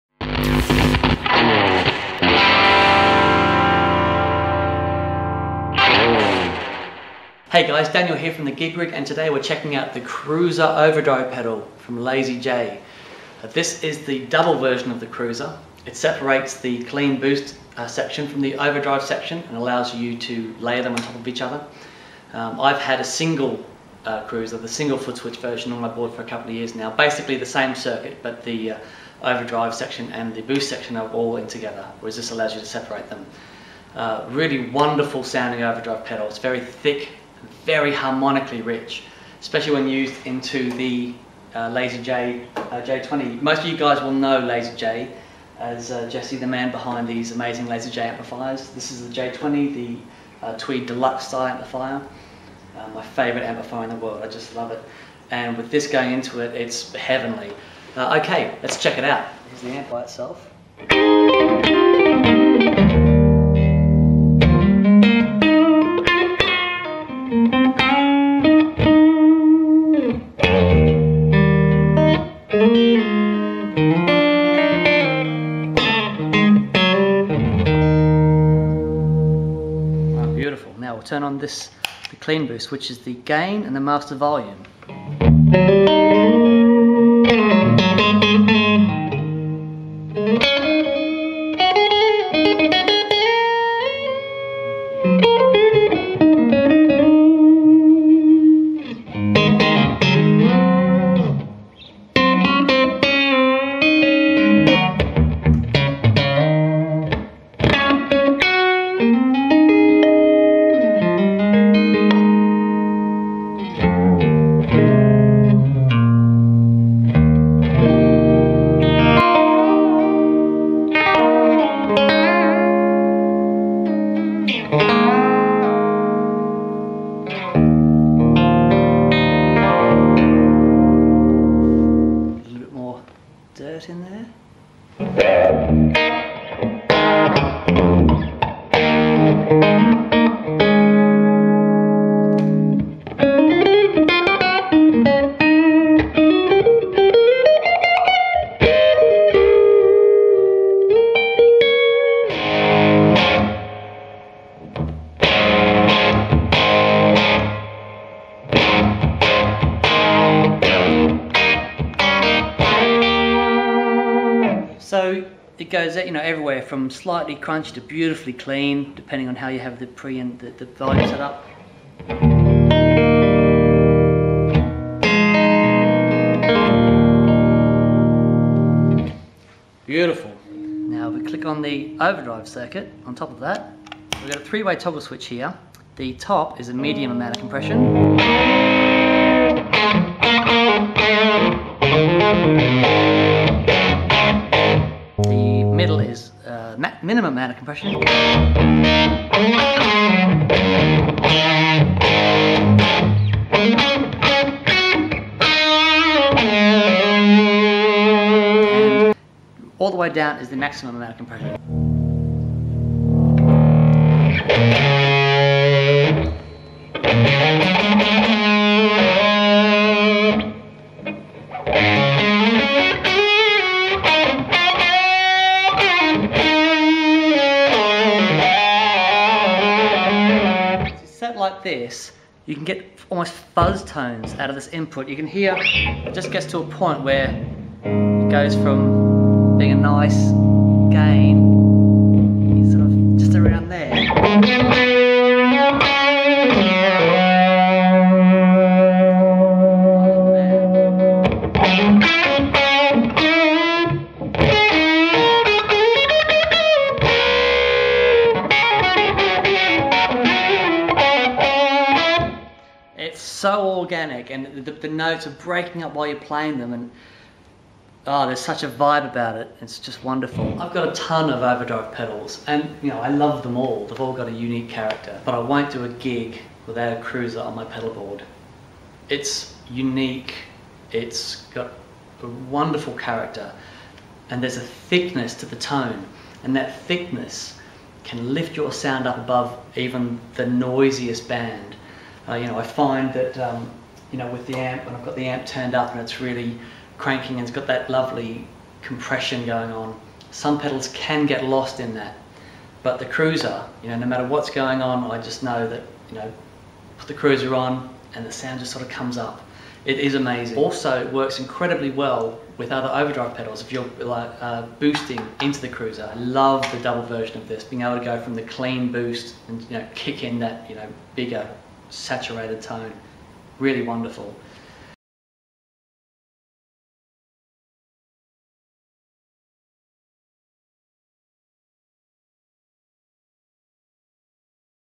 LazyJ Cruiser Overdrive Booster. Featuring LazyJ J20 with Fender Telecaster
The wonderful LazyJ Cruiser Deuce. Completely unique and a definite 'must try' if you're looking for a toneful flexible Overdrive booster.